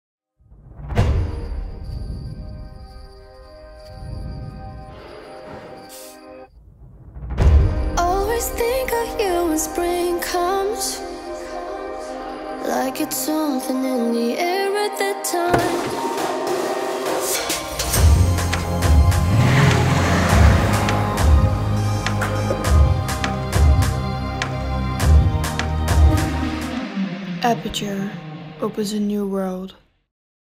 Aperture: opens a new world (Perfume Commercial)
Aperture: opens a new world.

Parfum commercial gemaakt in opdracht van het Grafisch Lyceum Rotterdam.